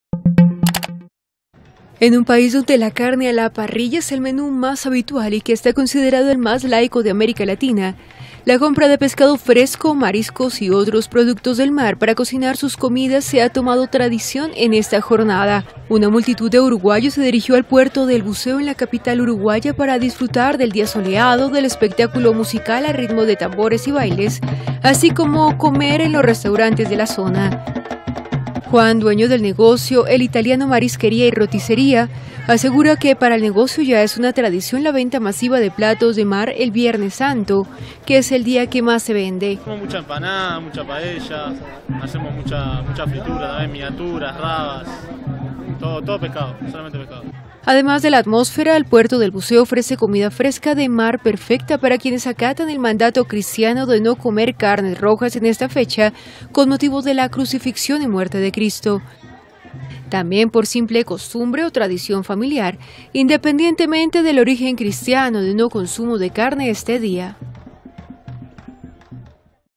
Uruguayos se vuelcan por el pescado al puerto del Buceo en Viernes Santo
Montevideo, 14 abr (EFE).- (Imágenes: Federico Anfitti) Una multitud de uruguayos se dirigió al puerto del Buceo, en Montevideo, para comprar pescados y mariscos con los que preparar sus comidas, ya sea por respeto a la tradición católica del Viernes Santo o por costumbre.